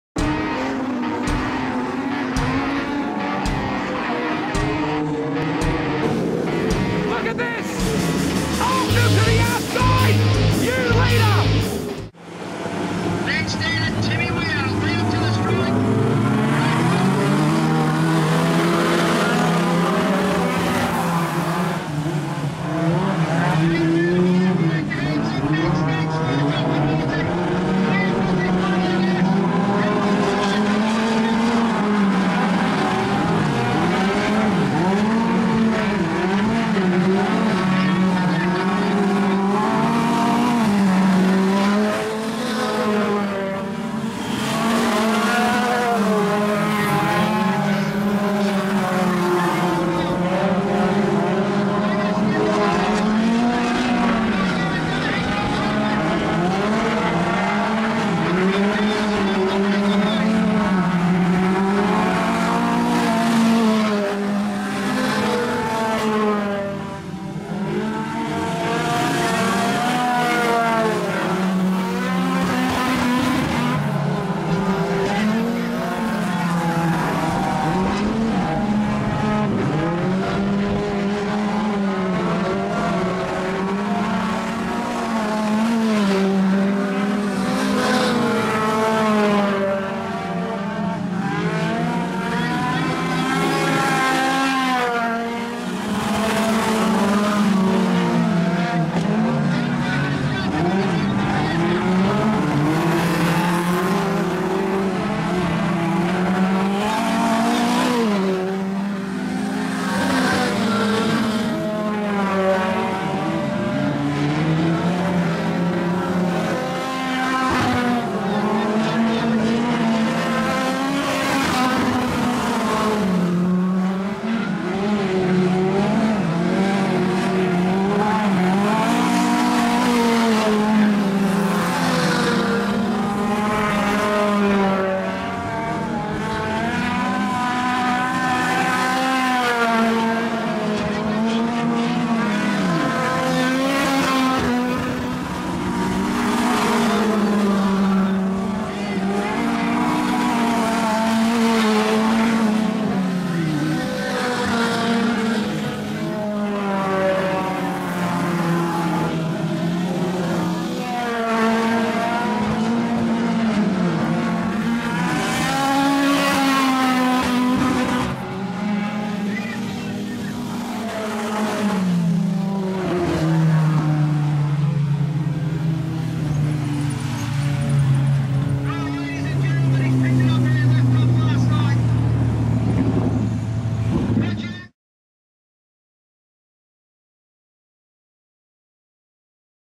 National 4's - Heat 7 - Carina Speedway - 27.04.16
This is Heat 7 of the National 4's held at Carina Speedway, Bundaberg on the 26.04.16